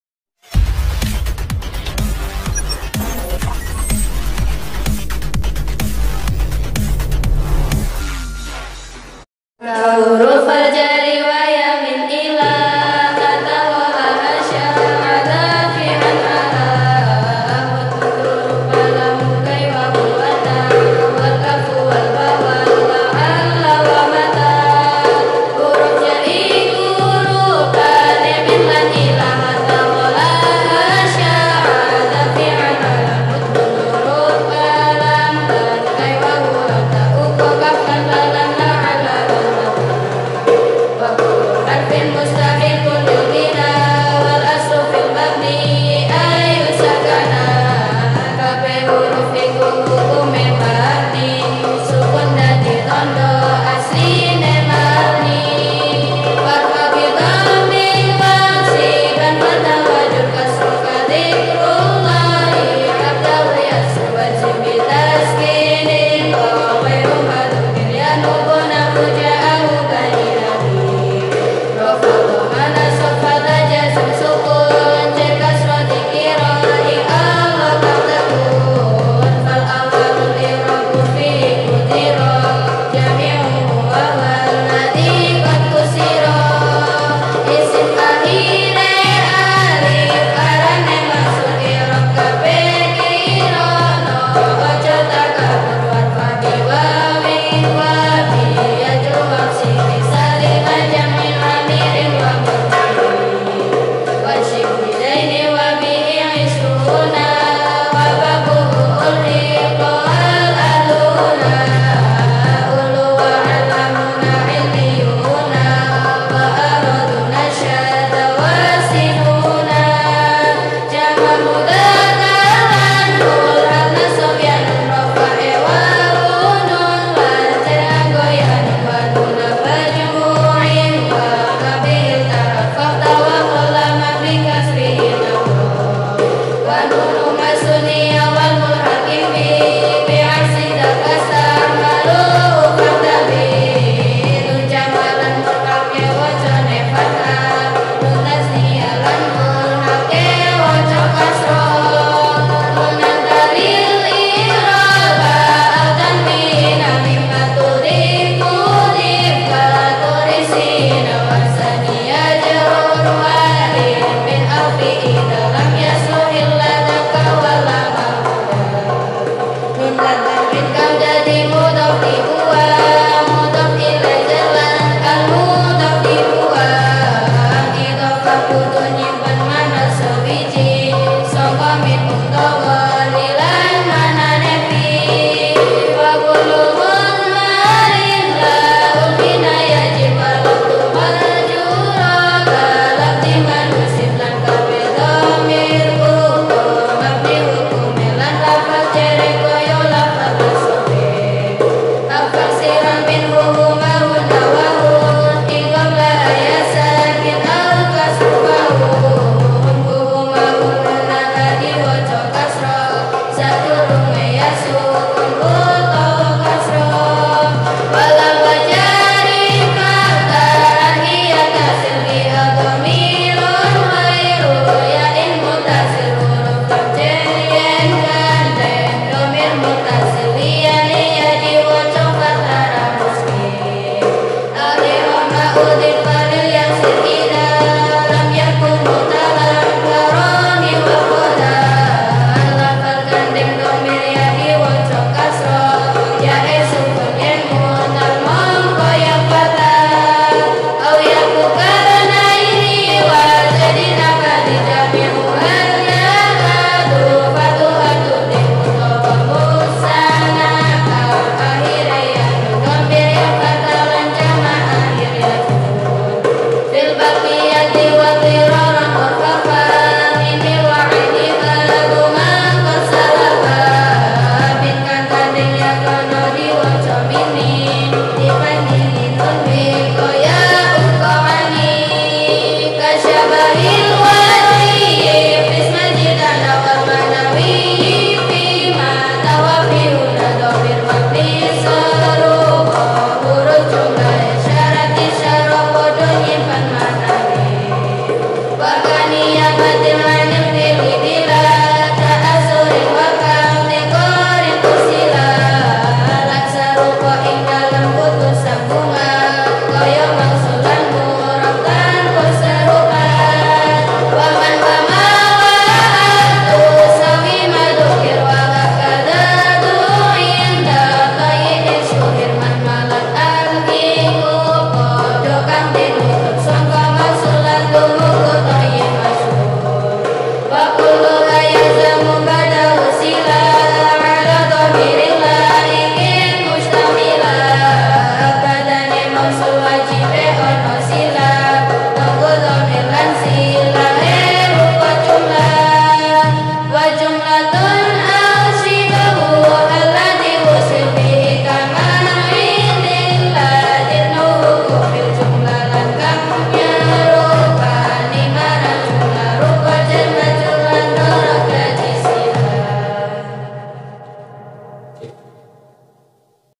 🎈full lalaran amtsilati jilid 1 santri putri _ PP Nurusaathi' Darul Falah
lalaran amtsilati santri putri pp Nurusaathi' Darul falah banjarnegra  full jilid 1 merdu